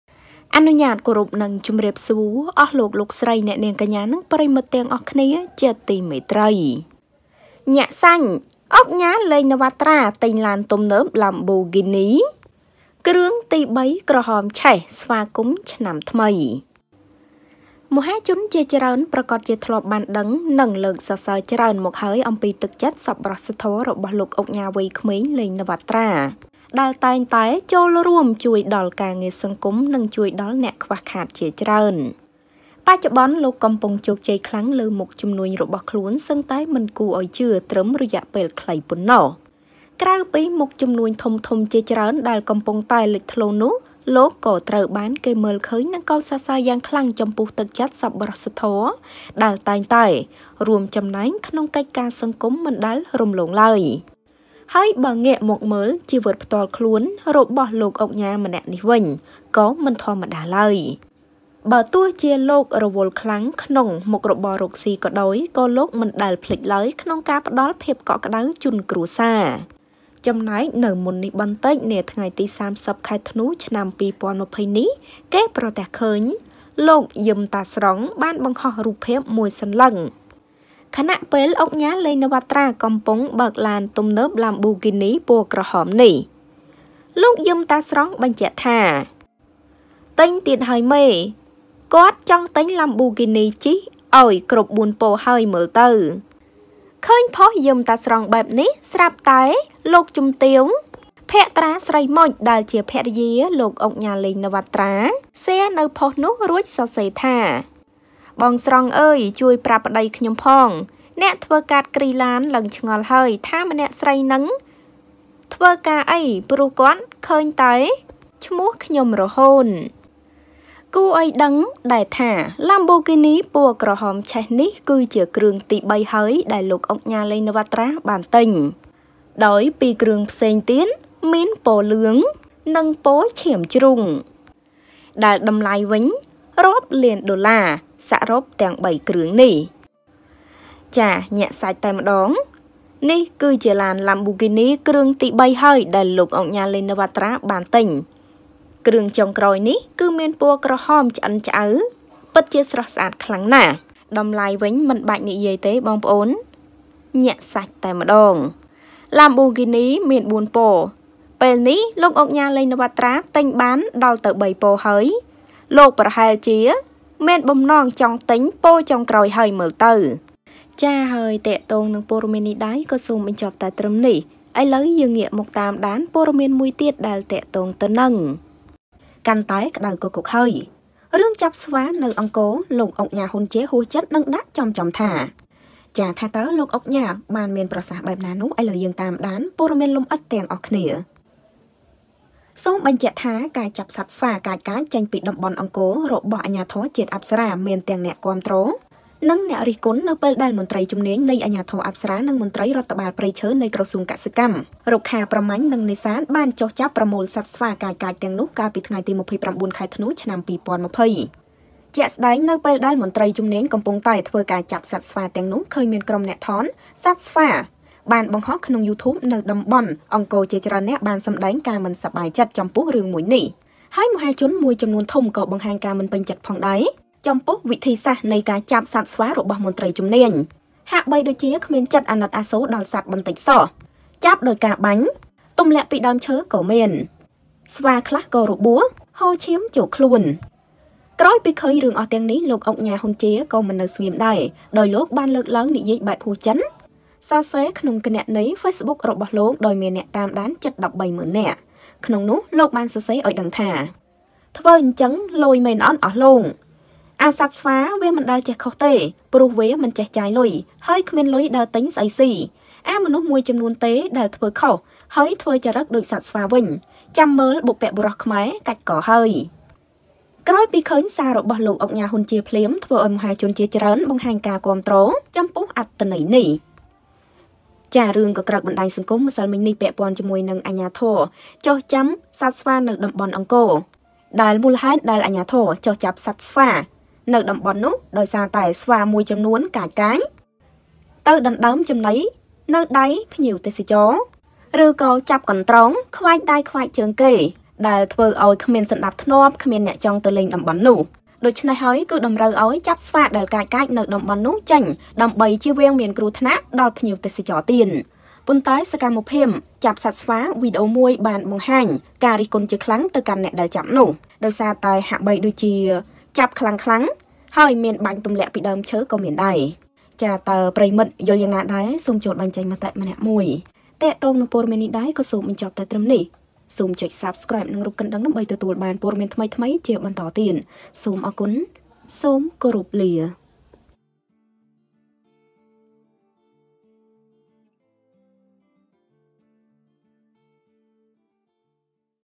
ញាក់សាច់ ឧកញ៉ា ឡេង ណាវ៉ាត្រា ទិញឡាន ទំនើប Lamborghini គ្រឿងទី ៣ ក្រហមឆេះស្វាគមន៍ឆ្នាំថ្មី
សូមអធ្យាស្រ័យ

ចំពោះការមិនបានតប ខំមិនបានគ្រប់គ្នា

Ms Socheata